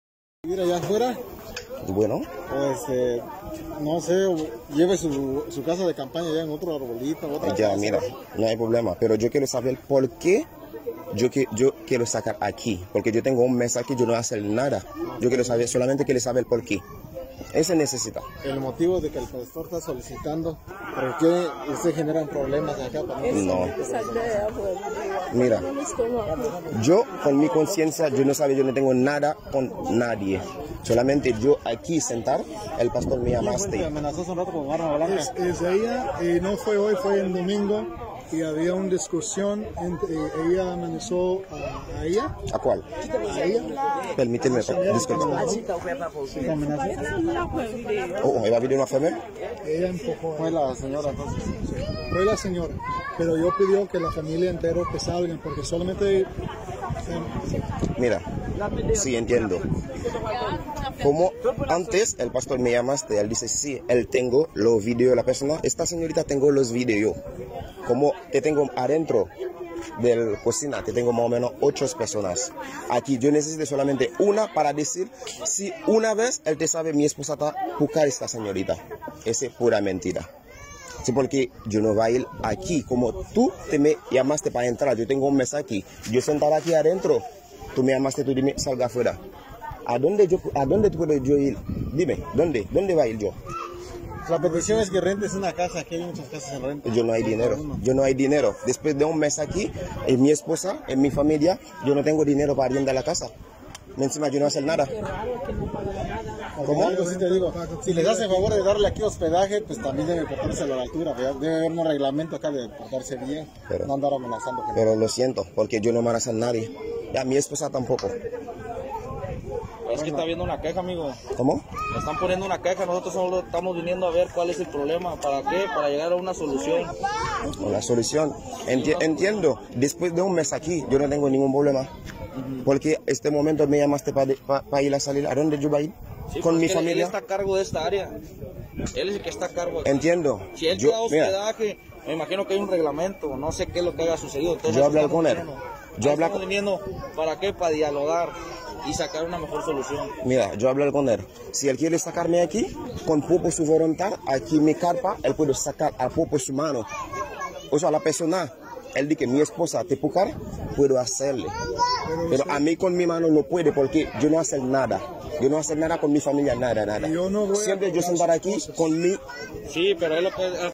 🔴Como delincuentes, es el trato que les dan a los migrantes en Reynosa
A pesar de vivir en condiciones infrahumanas y que no se ve un peso de los 21 millones de pesos que aprobó Carlos Peña y el cabildo de Reynosa para los migrantes, los tratan como delincuentes y los sacan de los refugios con policías.